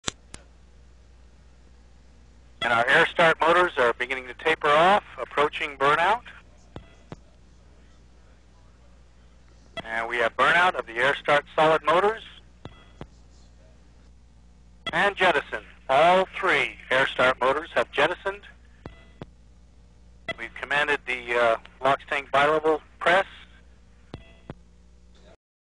2001 Dec 1: Delta II Launch of Jason and TIMED -- SRB Sep 2
A single aft-looking RocketCam captured this launch at dusk from the Vandenberg launch site in California.   Here the Delta's air-lit solid-rocket boosters are jettisoned.